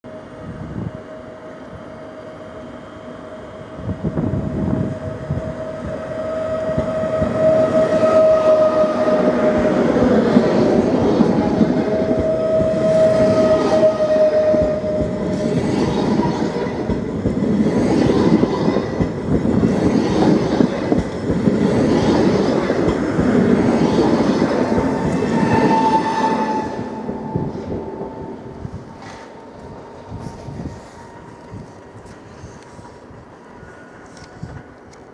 ED 161 - 007 z pociągiem IC 61190
Szklarska Poręba Górna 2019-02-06 -  ED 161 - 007 z pociągiem IC 61190 relacji Szklarska Poręba Górna - Białystok